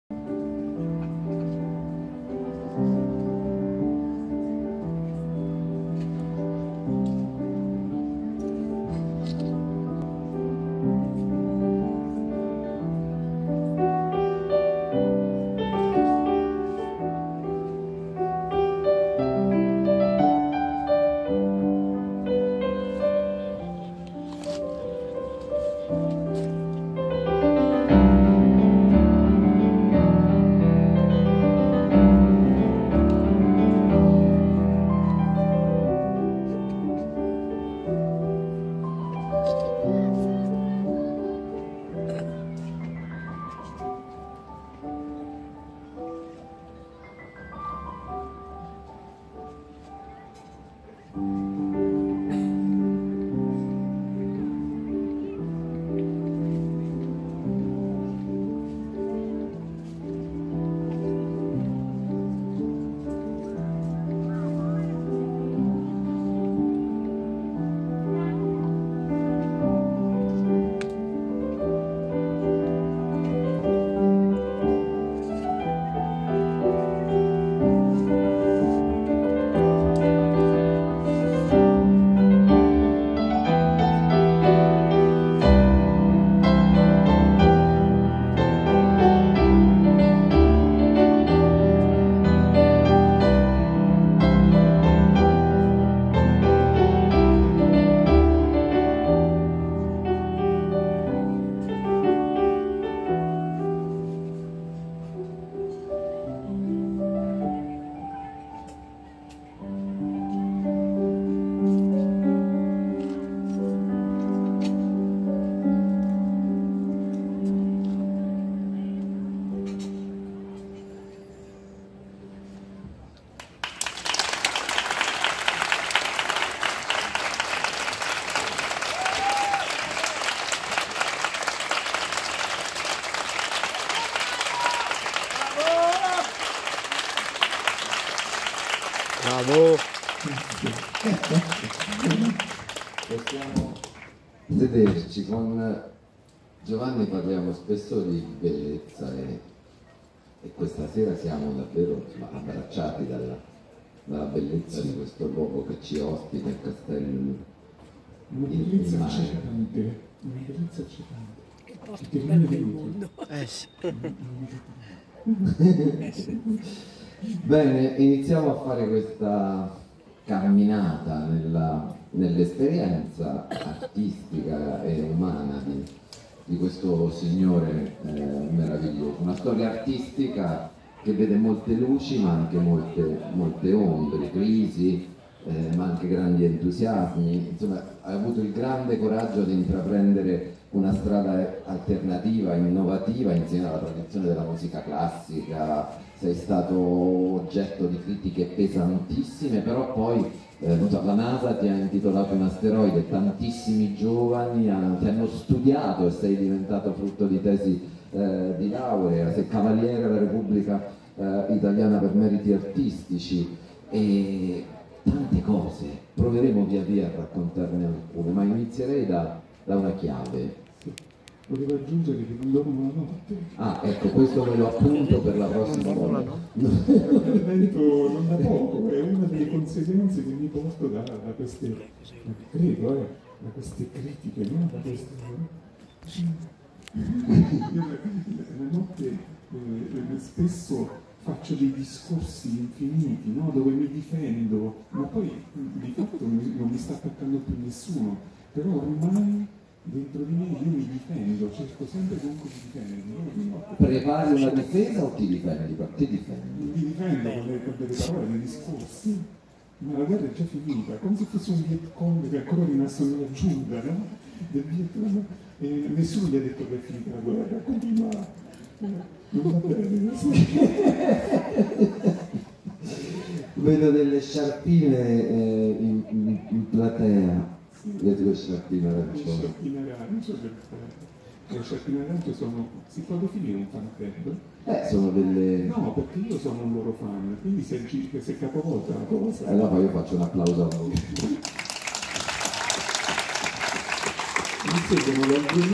Giovanni Allevi suona al Castello di Lerici
concerto del pianista di Ascoli Piceno. intervista di Pino Strabioli